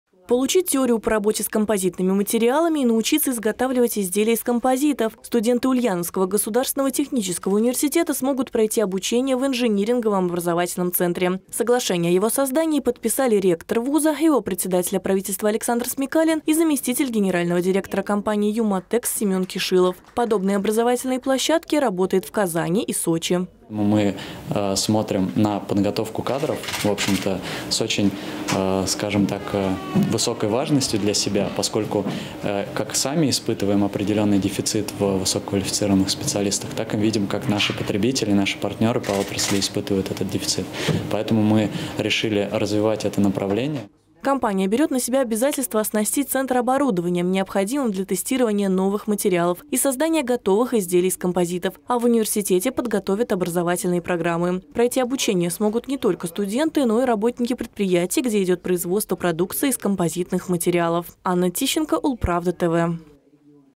Ульяновцев научат работать с композитами
На базе Ульяновского Политеха создадут инжиниринговый образовательный центр. Соглашение ВУЗ подписал с правительством области и компанией UMATEX Росатом.